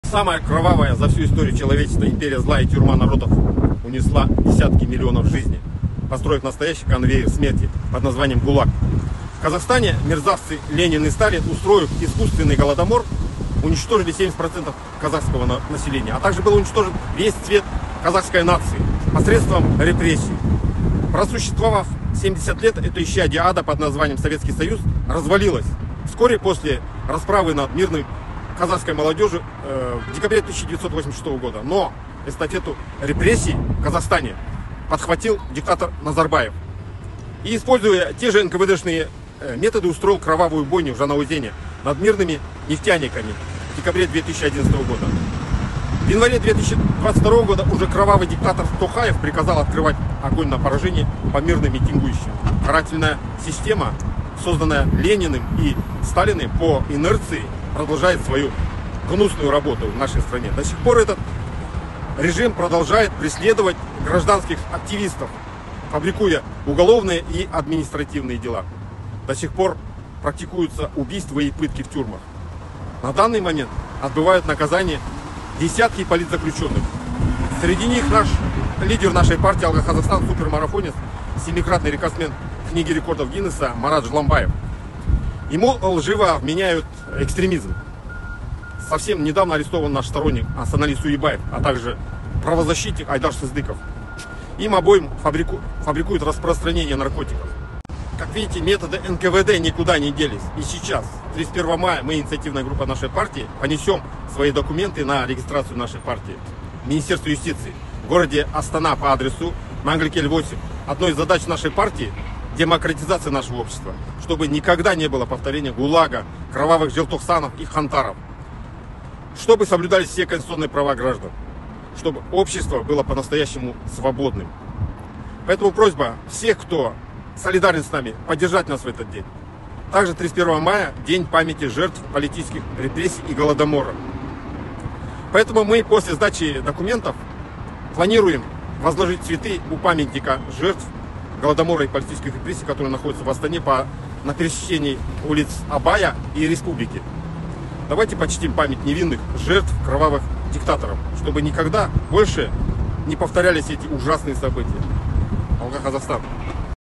ДЖАХИН АМАНГЕЛЬДЫ: 31 МАЯ В 14:00 ЧАСОВ ПОЧТИМ ПАМЯТЬ РЕПРЕССИРОВАННЫХ!
31 Мая в Казахстане – День памяти жертв политических репрессий и массового голода. Многострадальный народ Казахстана пережил немало страшных событий с массовыми репрессиями, среди которых события 1930-х, 1986-го, 2011-го, 2022-го годов.

В связи с этим, 31 МАЯ, в день 25-й подачи документов на регистрацию нашей оппозиционной партии «Алға Қазақстан» в Министерство юстиции, которая состоится в 12.00 часов дня по адресу:
-  Астана, ул.Мәңгіліг ел, 8,
после завершения процесса предоставления нами документов, МЫ ИДЕМ К ПАМЯТНИКАМ ЖЕРТВ РЕПРЕССИЙ И ГОЛОДОМОРА, чтобы почтить их память и возложить цветы.

К сожалению, политические репрессии происходят и сейчас, а память о жертвах событий 2011 года в Жанаозене и январских событий 2022 года свежа и кровоточит по сей день. Поэтому, ПРИЗЫВАЕМ ВСЕХ КАЗАХСТАНЦЕВ 31 МАЯ ПРИЙТИ ПОДДЕРЖАТЬ нашу очередную ПОПЫТКУ РЕГИСТРАЦИИ единственной реально народной оппозиционной партии «Алға Қазақстан» к Министерству Юстиции в Астане, а в остальных городах и областных центрах в это же время к зданиям Департаментов и Управлений Юстиции, а затем ПРОЙТИ ВМЕСТЕ С НАМИ К ПАМЯТНИКАМ ЖЕРТВ ГОЛОДОМОРА.

Почтить память жертв и возложить цветы - это не просто формальный жест, а наш долг и ответственность перед теми, кто пострадал в этих трагических событиях! Это наша возможность показать власти, что мы помним и ОСУЖДАЕМ ВИНОВНИКОВ ТЕХ СОБЫТИЙ, от рук которых страдали и погибли тогда безвинные наши соотечественники!

@mukhtarablyazov (инстаграм)
@ablyazovmk10 (телеграм)